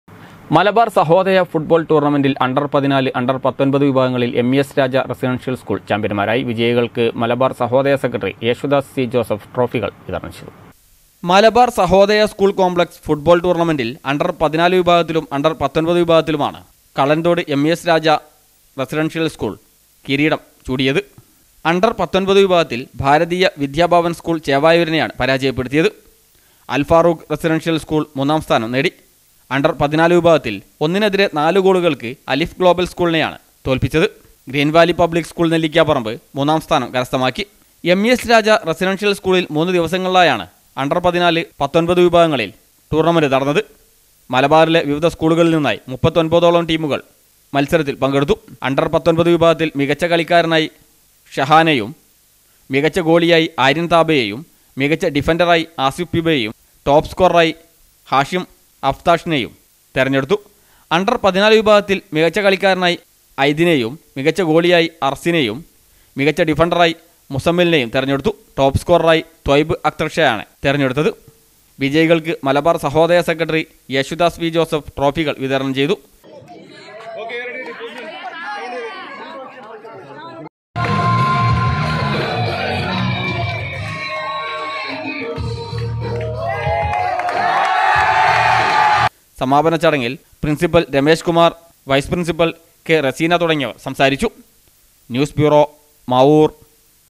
മലബാർ സഹോദയ ഫുട്ബോൾ ടൂർണ്ണമെൻ്റിൽ അണ്ടർ 14, 19 വിഭാഗങ്ങളിൽ എം ഇ എസ് രാജ റസിഡൻഷ്യൽ സ്കൂൾ
മലബാർ സഹോദയ ഫുട്ബോൾ ടൂർണ്ണമെൻ്റിൽ അണ്ടർ 14, 19 വിഭാഗങ്ങളിൽ എം ഇ എസ് രാജ റസിഡൻഷ്യൽ സ്കൂൾ ചാമ്പ്യൻമാരായി